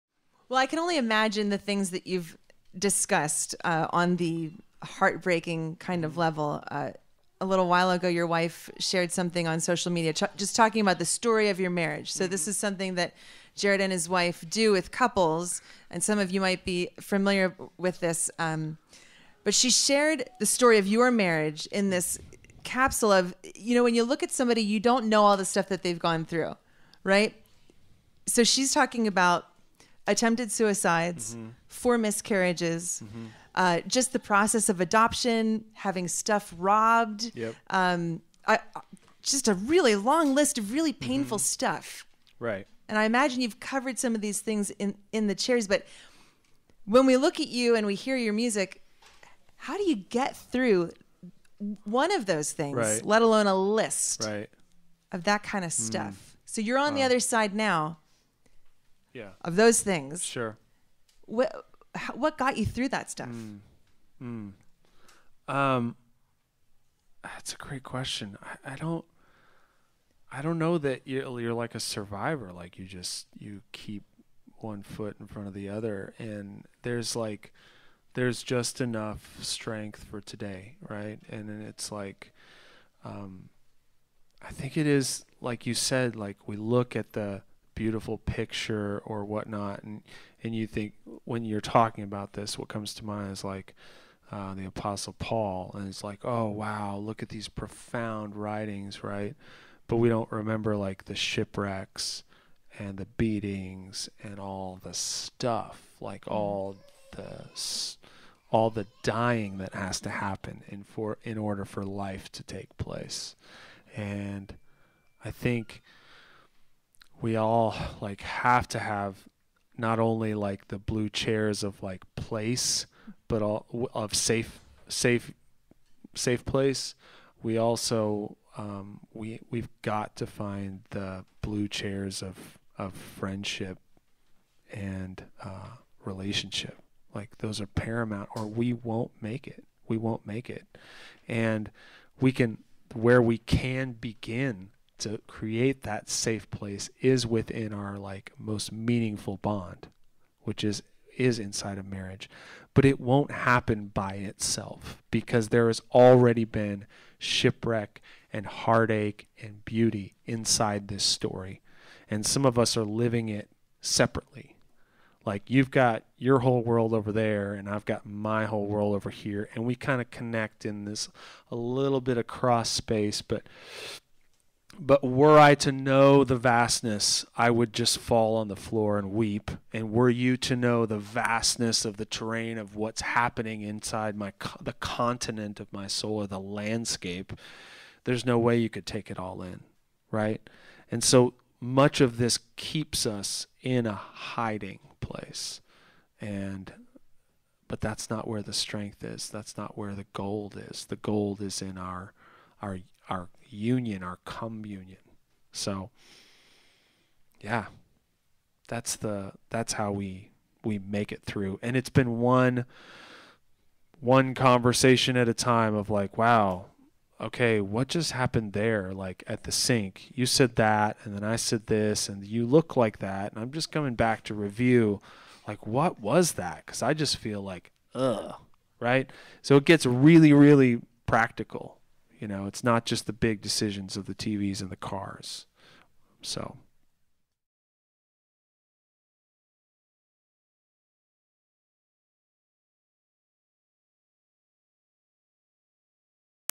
Jared Anderson: Getting Through Difficulty
Jared Anderson talks about how he and his wife and gotten through some of the most difficult times of their lives.

From Jared Anderson's Live from the Junction Center performance on November 23, 2019.

FM 90.3 - WJTL - CHRIST, COMMUNITY, MUSIC